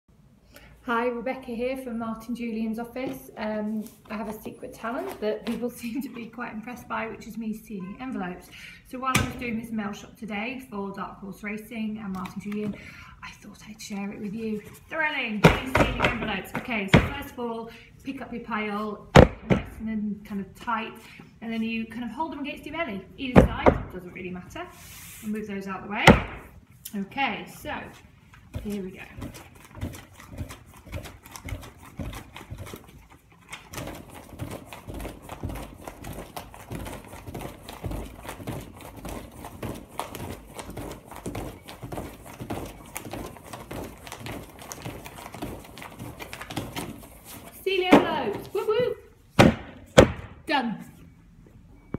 Rebecca Julian Dixon sealing envelopes
Rebecca would like to share her hidden talent (tee hee). Sealing envelopes super fast!